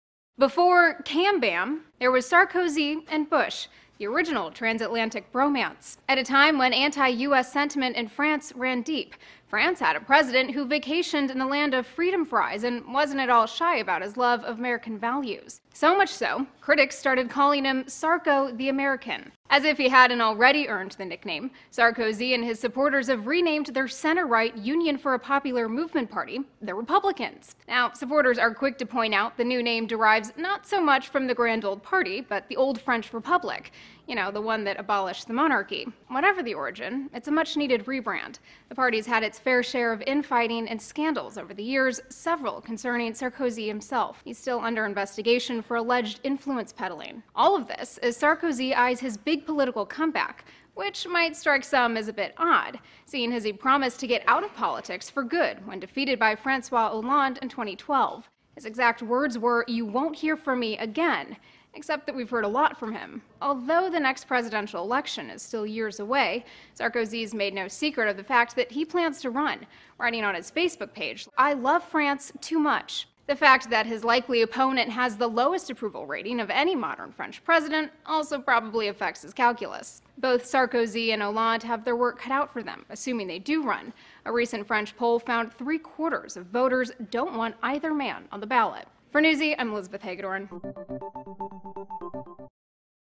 How France Got Its Own Republican Party
As part of former French President Nicolas Sarkozy"s return to the political scene, he"s rebranded his UMP party.